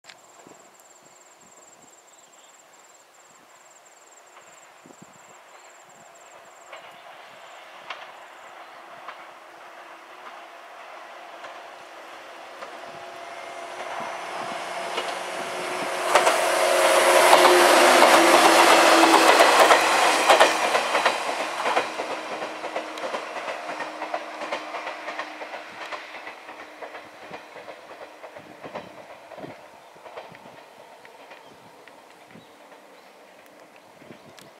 【紀南代走】105系（4扉車・片側103系顔）｜紀伊田辺方面への回送
JR西日本きのくに線（紀勢本線）。和歌山方面から紀伊田辺方面の回送列車として走る4扉車で和歌山方面は元車の雰囲気を残す103系顔の105系電車。2019年11月12日。和歌山県日高郡印南町。